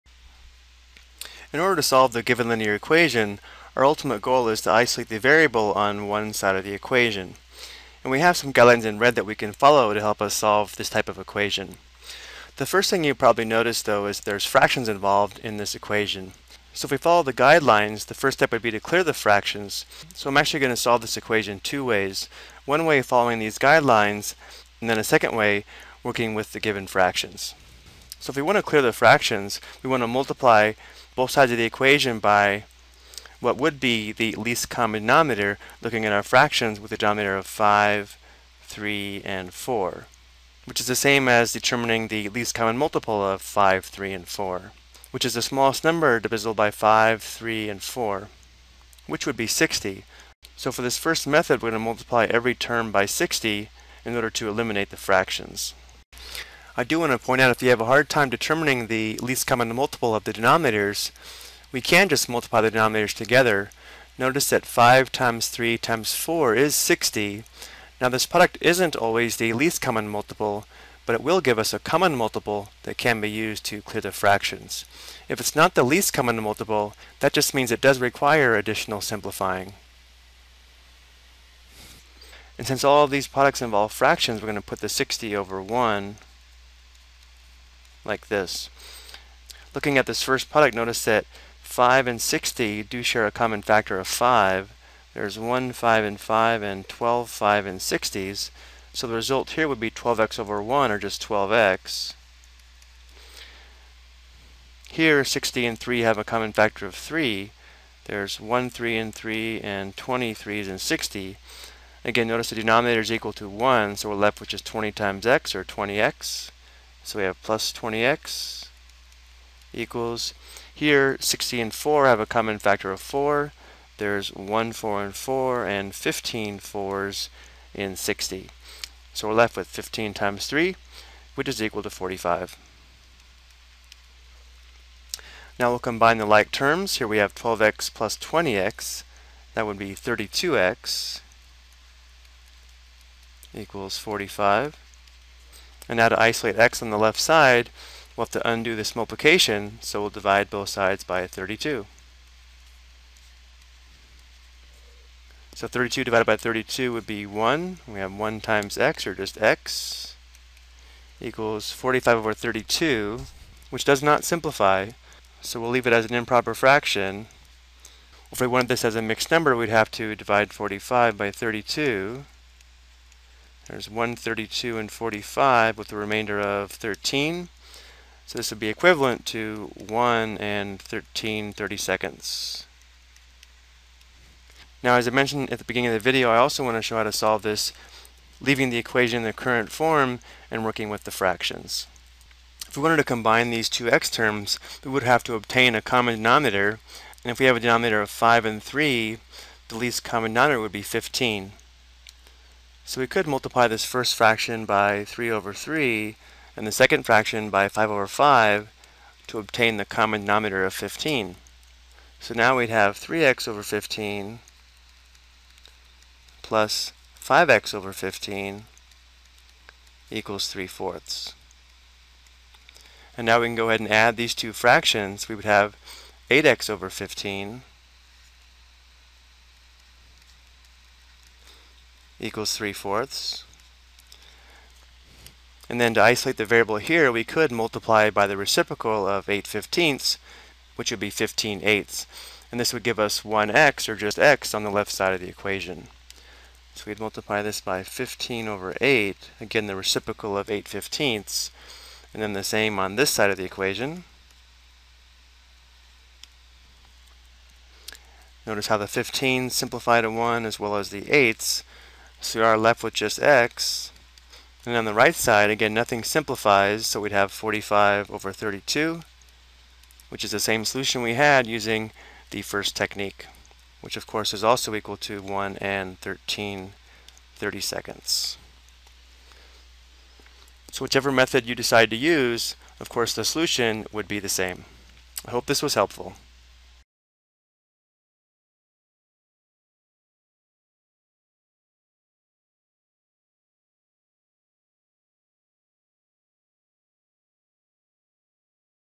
Ex: Solve an Equation with Fractions
This video provides an example of how to solve a linear equation in one variable with fractions.